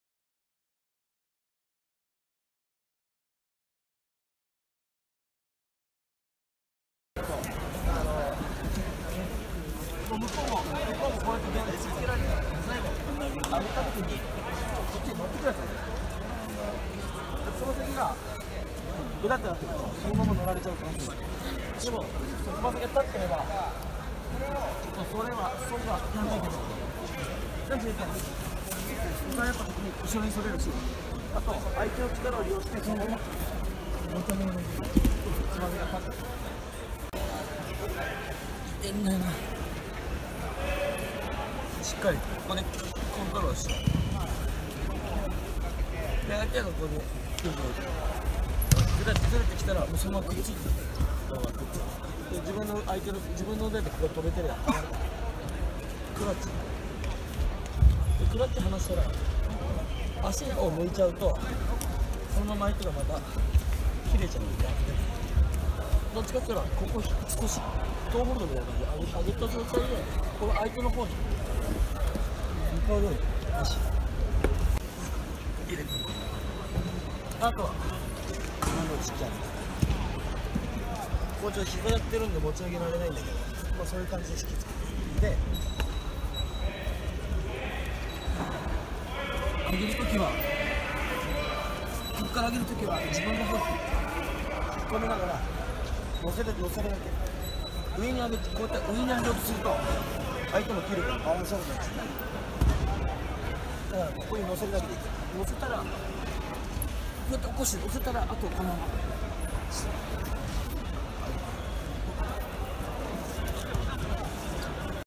2007年世界選手権2位笹本選手のリフト指導
説明　レスリング指導に役立てばと思います。